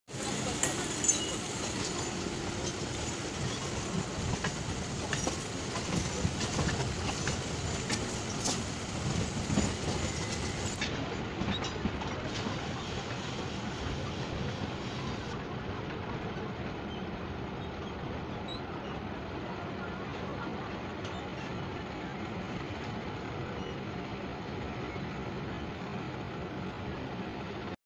local train of West Bengal eastern Railway of India dumdum local train
video taken on 06/04/2023 at Dumdum junction from Habra local